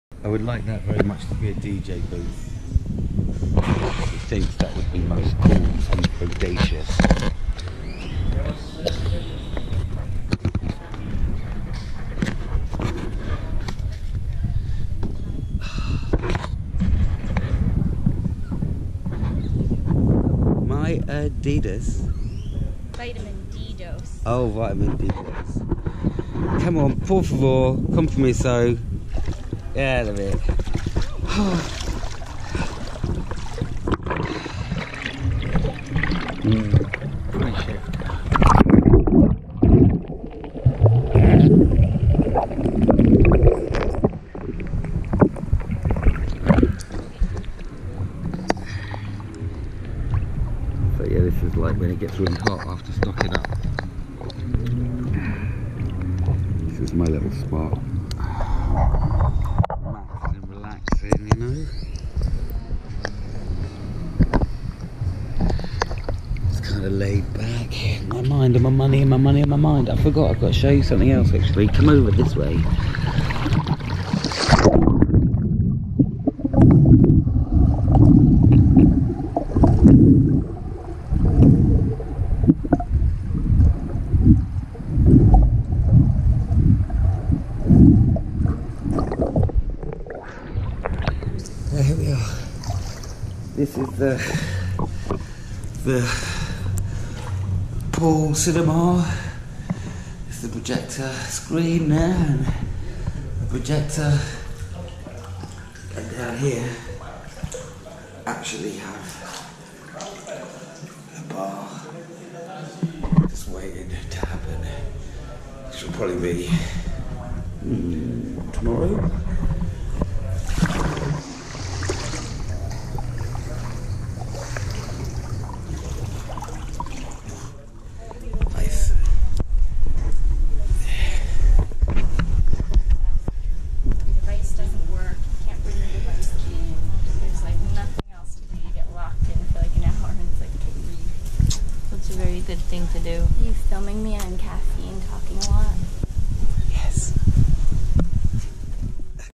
HulaKai Pool
Not only does Hula Kai have a pool with a view, it also contains a secret underwater cinema!